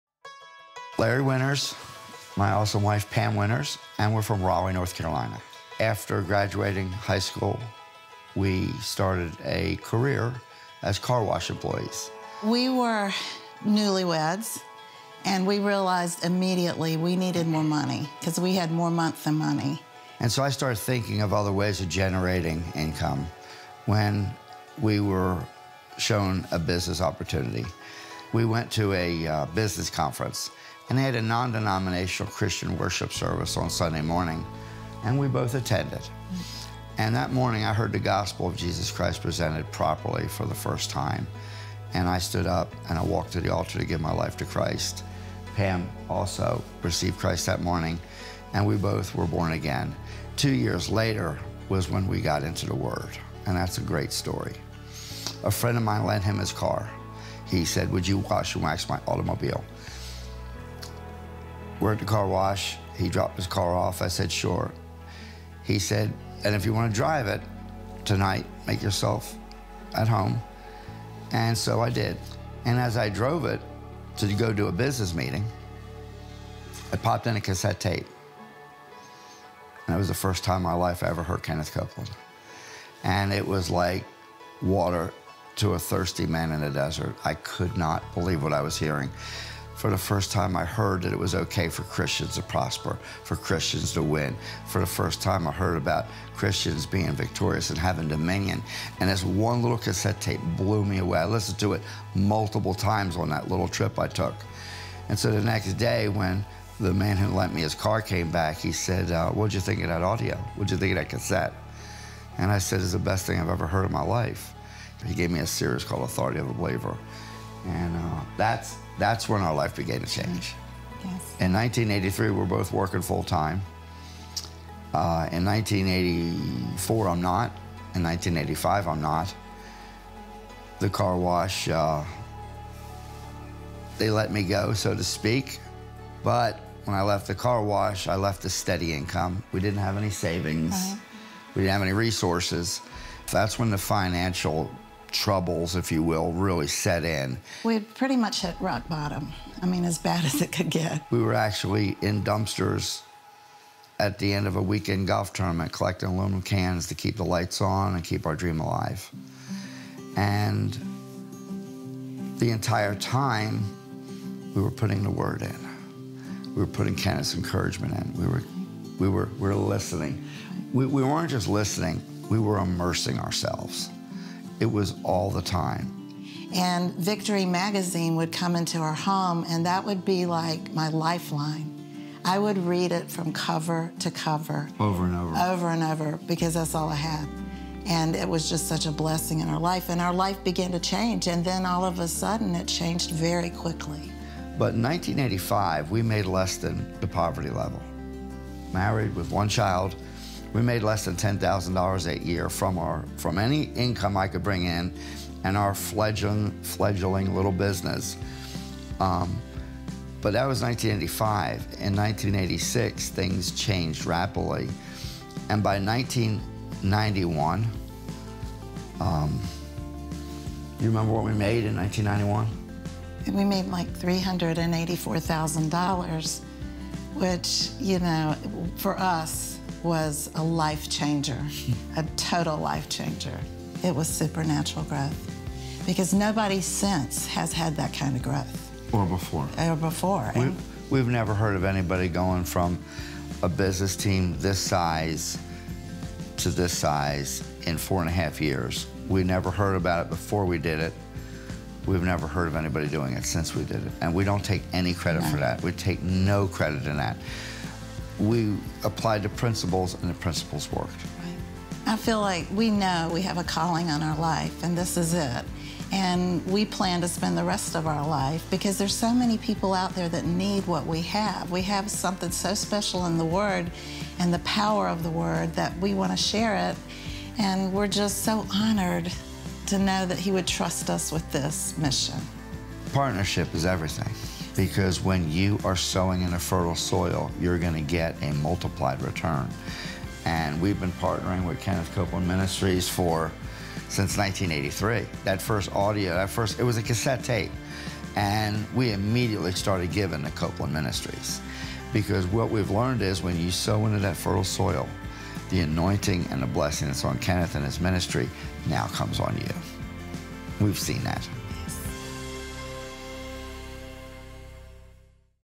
From Rock Bottom to Victory in Finances—KCM Partner Testimony
Longtime KCM Partners Larry and Pam Winters were once newlyweds and newfound believers with a business dream in their hearts, but they were met with financial problems right off the bat.

A couple of years into their walk with the Lord, Larry’s friend lent him his car to be washed, letting him borrow it for the night. Driving the car on the way to a business meeting, Larry decided to pop in one of his friend’s cassette tapes. That was the first time he had ever heard Kenneth Copeland speak. It was also the first time he had ever heard that it was OK for Christians to live prosperous, victorious lives. He couldn’t believe what he was hearing!

When Larry returned the car, his friend gave him the Kenneth Copeland series, Authority of the Believer, and that is when Larry and Pam’s lives really began to change. Over the next several years, Larry lost his job and steady income. Larry and Pam were making below the poverty line and were resorting to collecting aluminum from dumpsters to make ends meet. They had hit rock bottom, but they continued to immerse themselves in God’s Word, in Kenneth Copeland’s teachings, and in the Believer’s Voice of Victory magazine.

It wasn’t long before their lives began to change rapidly. Their fledgling of a business experienced a supernatural growth unheard of even today!

Throughout their financial struggles, Larry and Pam were faithful in their sowing and continued to apply God’s principles in their lives—now they’re living the prosperous, victorious life God promises!